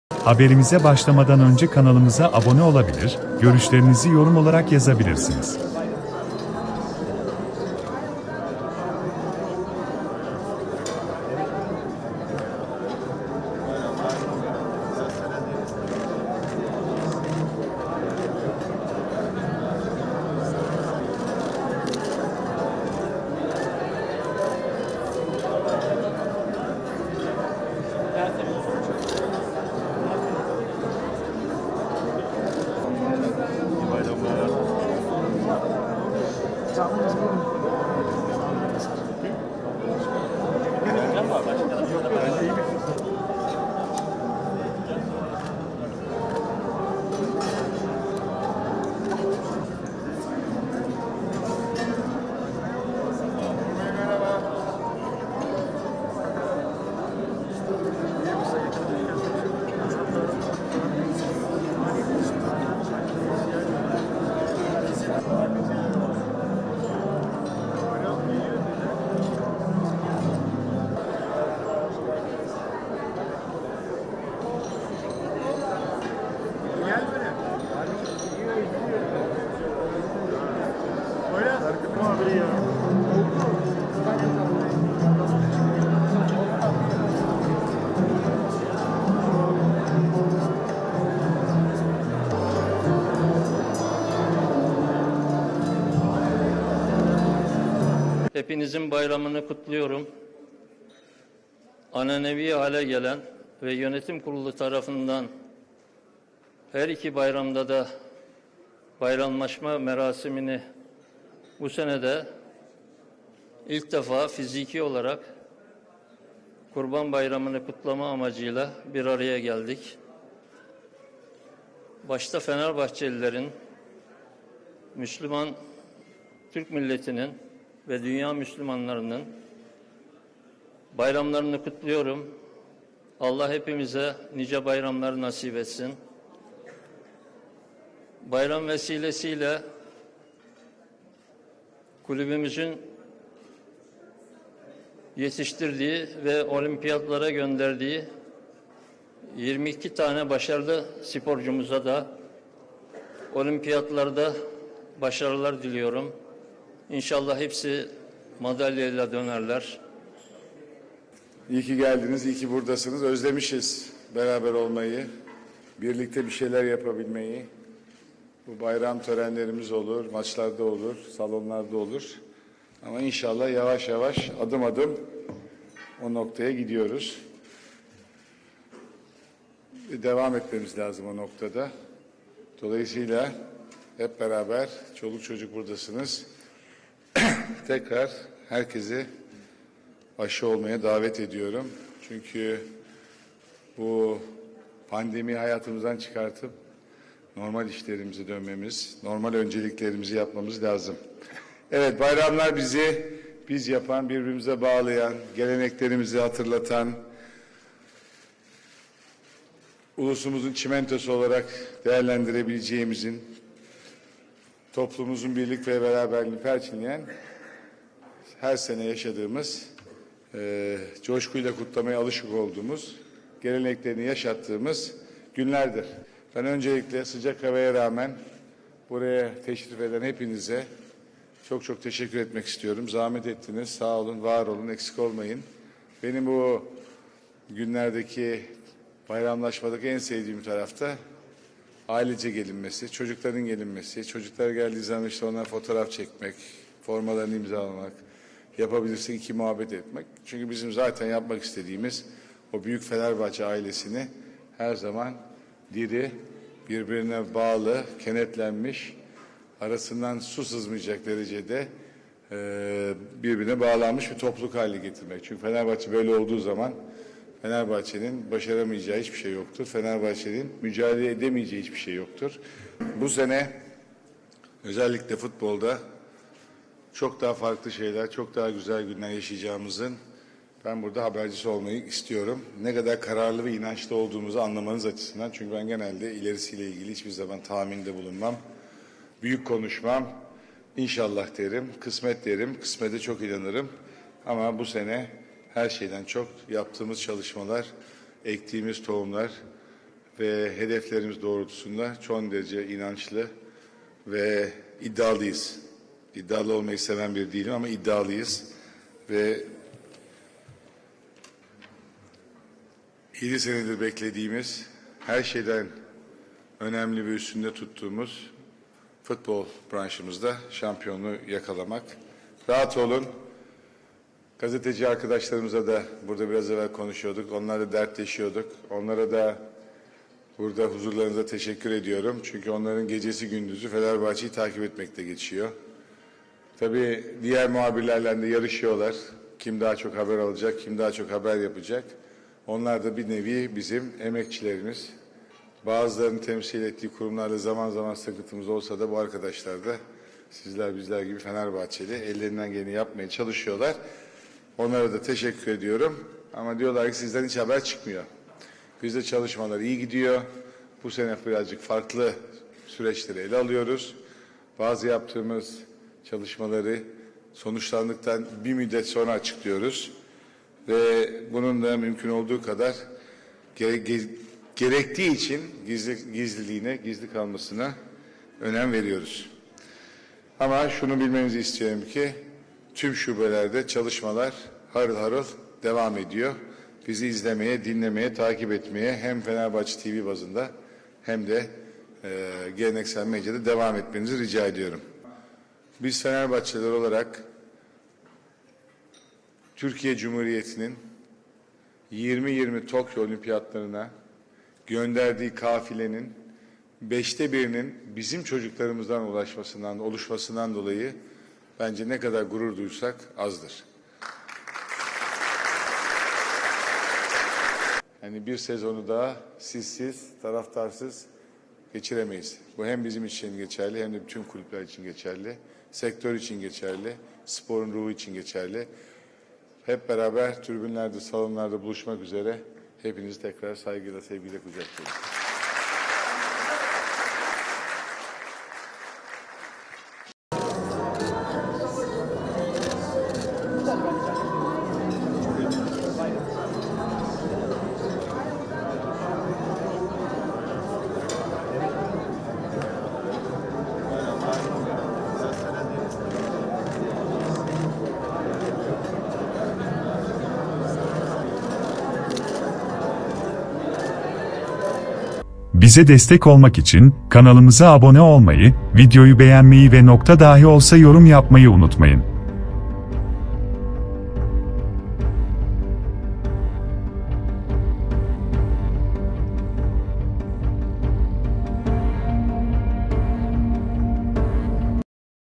SON DAKİKA! Fenerbahçe Başkanı Ali Koç'tan Şampiyonluk Açıklaması! 7 Senedir Şampiyon...
Fenerbahçe Kulübünün, geleneksel bayramlaşma töreni başkan Ali Koç'un katılımıyla gerçekleştirildi.
Fenerbahçe Faruk Ilgaz Tesisleri'nde Kurban Bayramı dolayısıyla gerçekleştirilen törene, başkan Koç'un yanı sıra yöneticiler Erol Bilecik, Ruşen Çakır, Burak Çağlan Kızılhan, Ahmet Ketenci, Selma Altay Rodopman ve Özgür Özaktaç katıldı.
Başkan Ali Koç, tek tek masaları gezerek üyelerle bayramlaştıktan sonra bir süre basın mensuplarıyla sohbet etti.
Törende ilk konuşmayı yapan Fenerbahçe Kulübü Yüksek Divan Kurulu Başkanı Vefa Küçük, sözlerine herkesin bayramını kutlayarak başladı.
Yeni sezon öncesi futbol takımına başarı dileğinde bulunan Küçük, olimpiyata gönderdikleri 22 sporcudan madalya beklentilerini de aktardı.
Başkan Ali Koç da katılımlarından dolayı üyelere teşekkür etti.